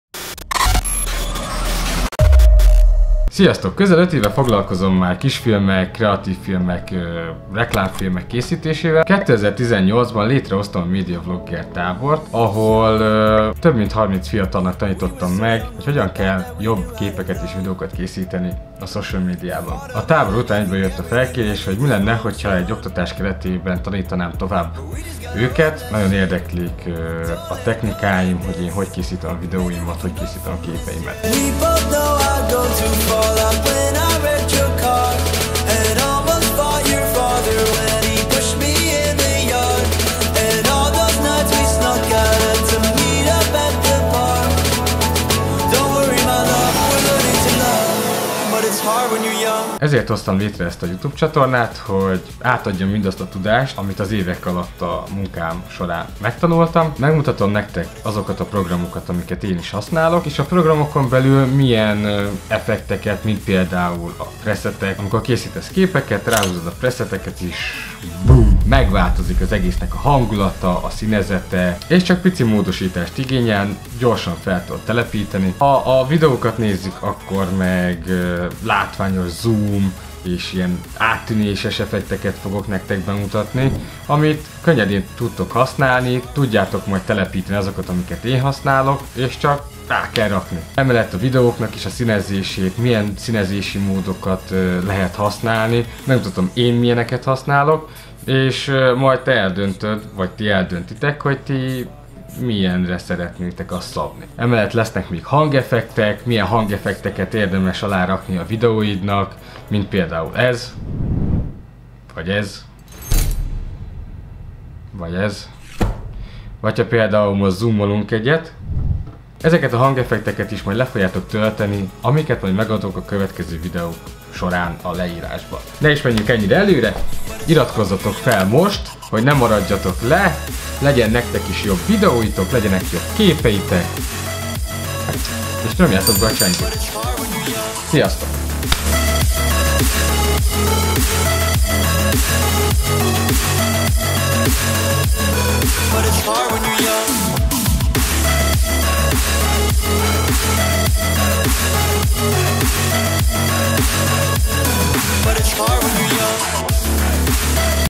Ez lesz 2019-ben a CSATORNÁN!
Ebben a videóban bemutatkozom nektek, és hogy mi a tervem a 2019-es évben! Tartsatok velem!

Ne maradj le a következő videókról, KÖVESD BE A CSATORNÁT, és Igyál egy finom TEÁT! :)